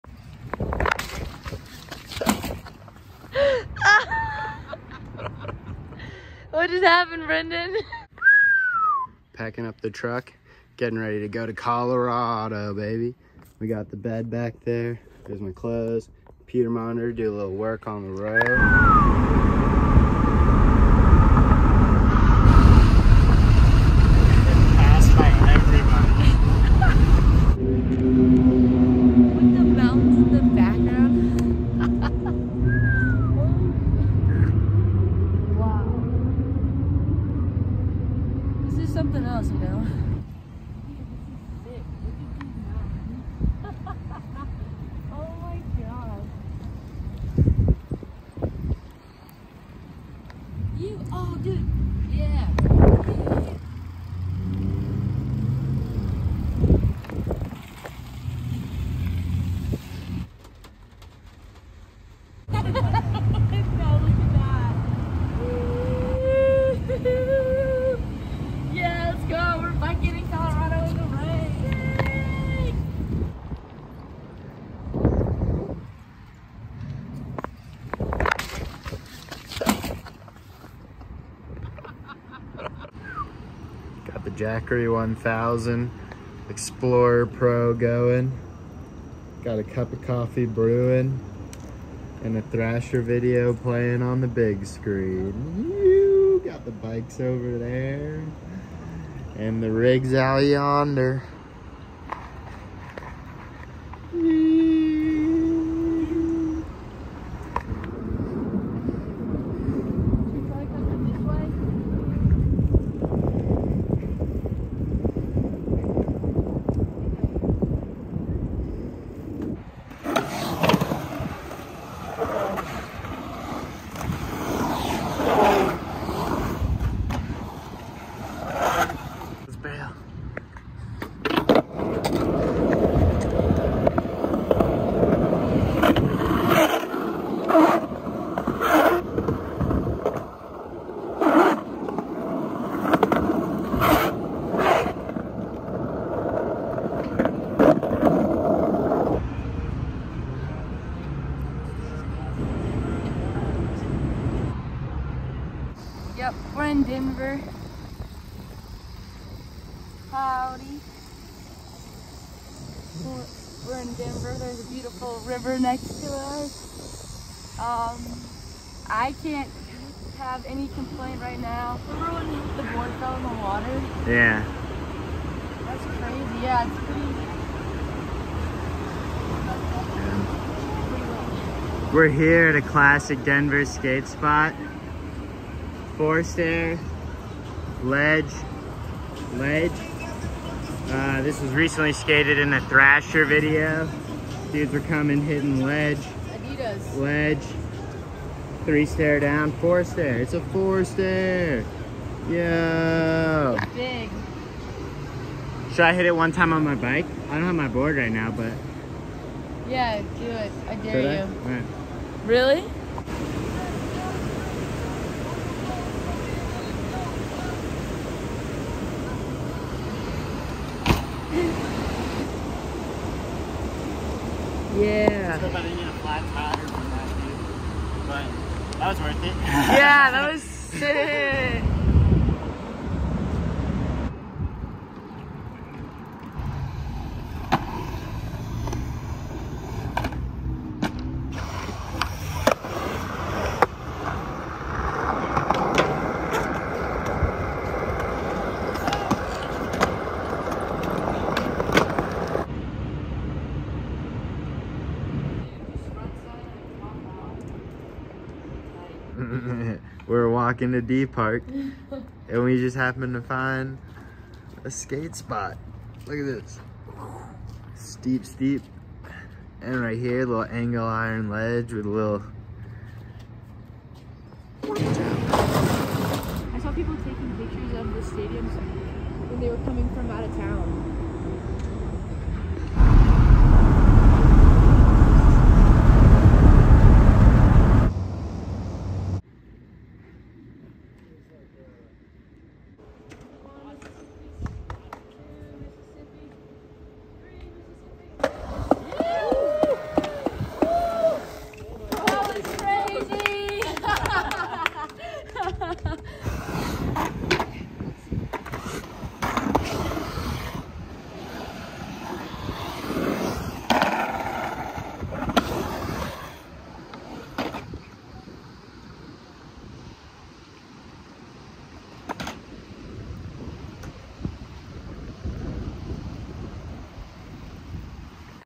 Denver Skatepark and Biking in Colorado
Packing Up the Truck Camper for an Epic Colorado Road Trip (Atlanta to Colorado)

Join me as I pack up my truck camping setup and hit the road from Atlanta, GA, heading west for an epic adventure through Colorado! Our first stop is Colorado Springs, where we skate at Memorial Skatepark and bike through the city. Then, it's off to Denver for some more shredding at Denver Skatepark, biking around the city, and catching Green Day live on their Saviors Tour!

Next, we explore the beautiful Flatirons in Boulder and keep the momentum going with more skating and biking. Finally, we end up in the mountain town of Nederland, where we take on the Nederland Skatepark before settling into the quiet mountain vibes.

Along the way, our trusty Jackery power bank keeps us charged up, making the journey smoother.

Memorial Skatepark (Colorado Springs)
Denver Skatepark (Denver)
The Flatirons (Boulder)
Nederland Skatepark (Nederland)

Notable Moments:
0:00- 0:18: Trip Intro
0:18- 0:27: Road Tripping
0:27- 0:40: Arriving in CO Springs
0:40- 1:23: Biking in CO Springs
1:23- 1:48: Jackery Power Bank
1:48- 2:29: Memorial Skatepark
2:29- 2:36: Colorado Night Life
2:36- 2:52: Biking in Denver
2:52- 3:52: Confluence Park, Denver
3:52- 4:12: Denver Skatepark
4:12- 4:35: Denver Skate Spots
4:35- 4:43: GREEN DAY!!!
4:43- 4:53: Flat Irons, Boulder
4:53- 5:28: Nederland Skatepark